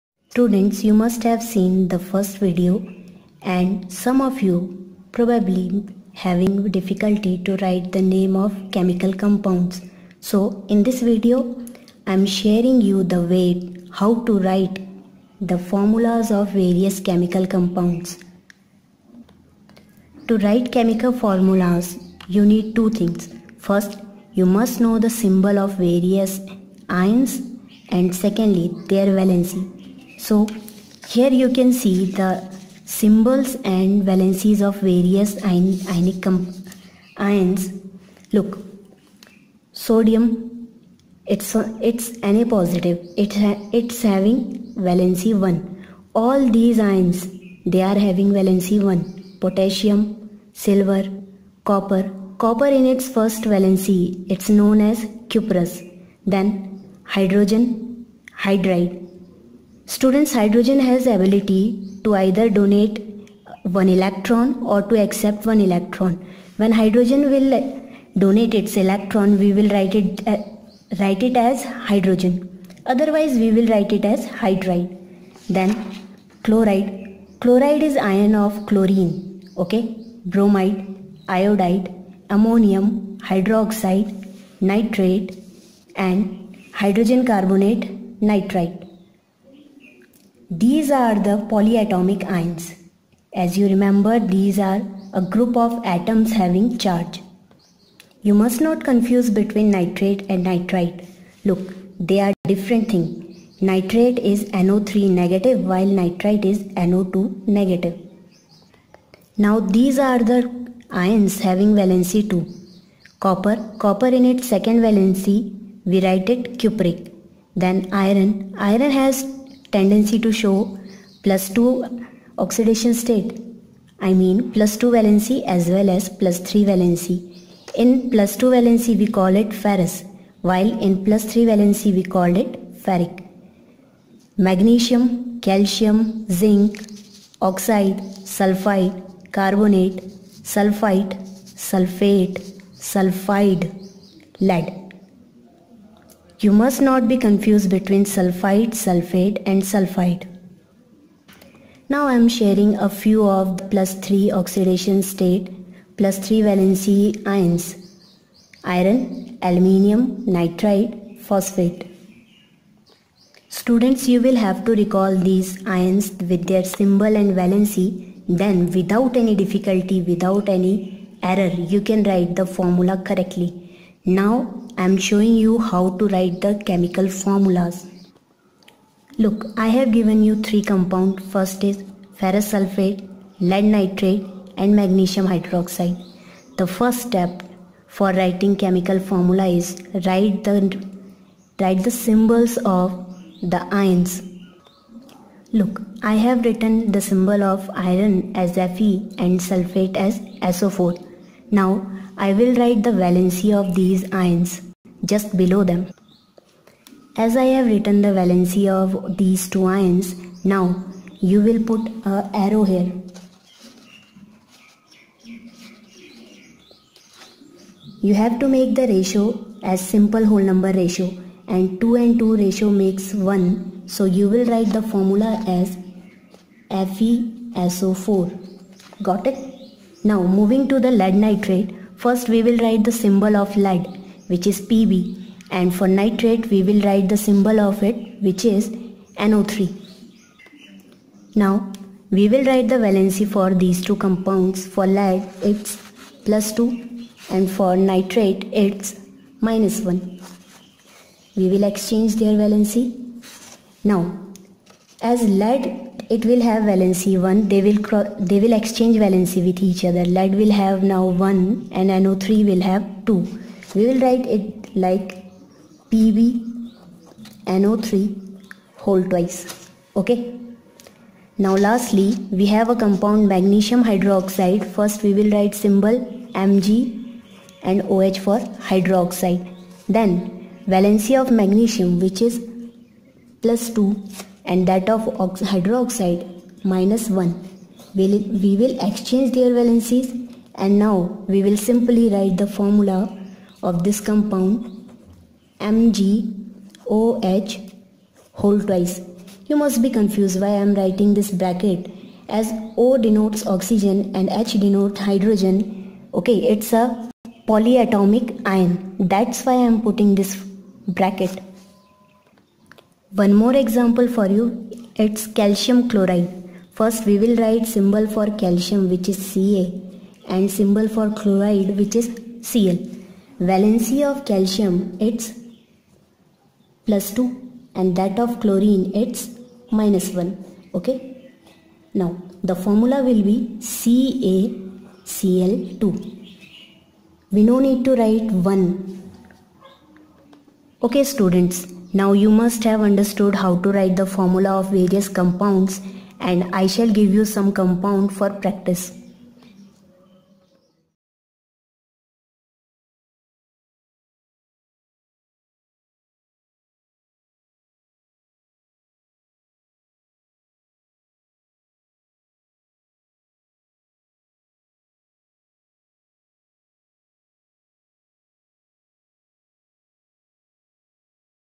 Writing chemical formula
A detailed explanation for writing chemical formula for various compounds